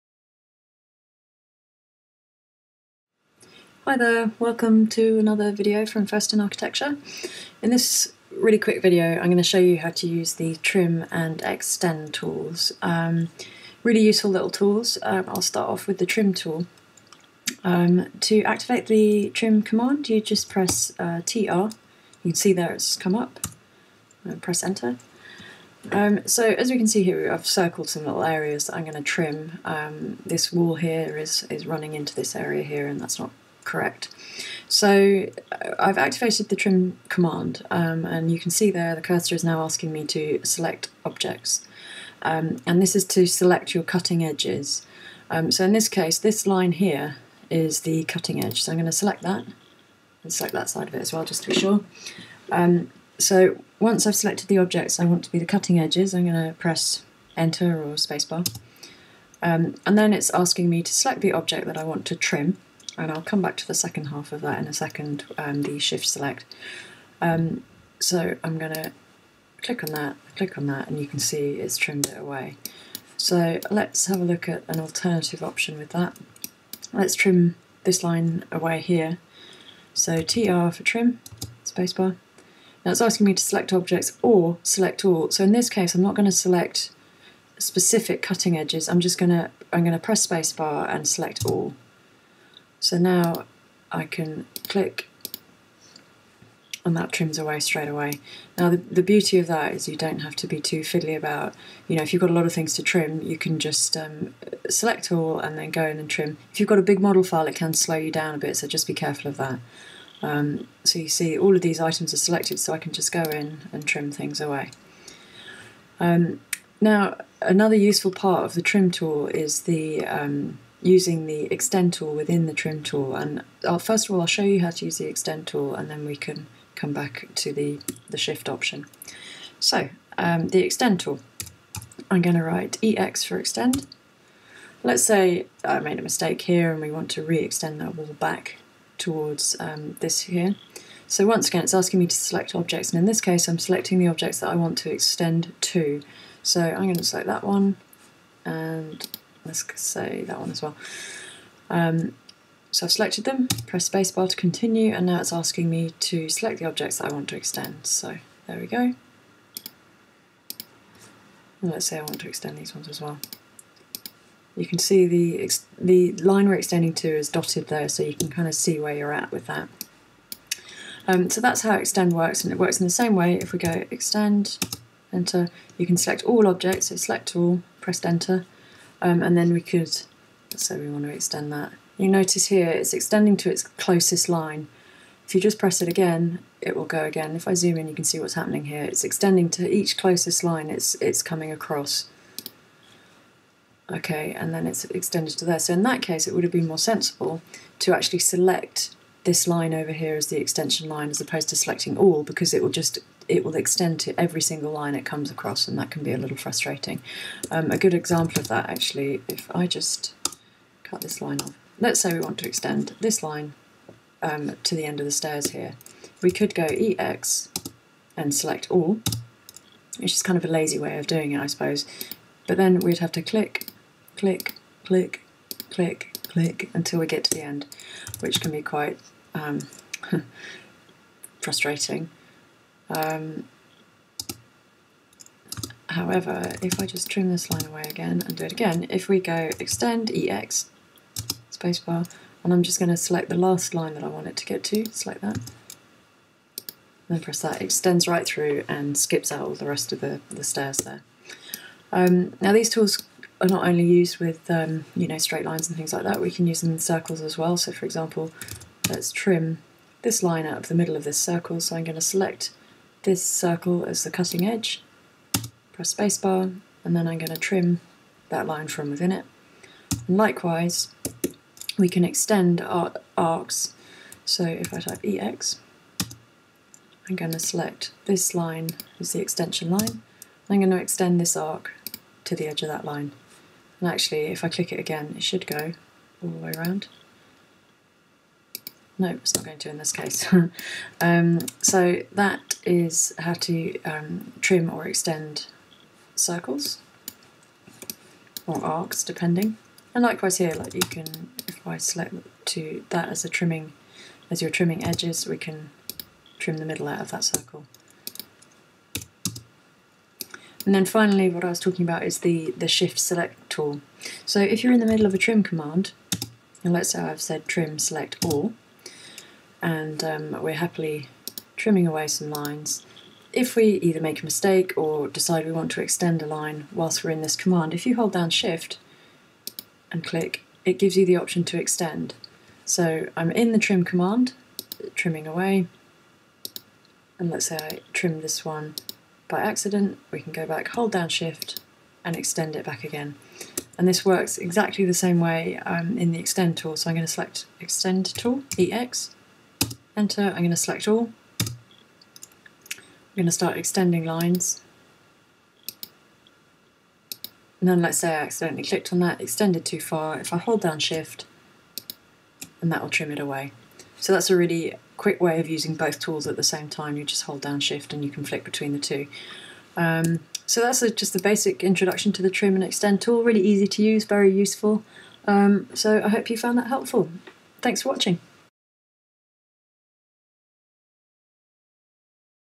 Quick Cad Tips | Trim and Extend
How to use the trim and extend commands in Autocad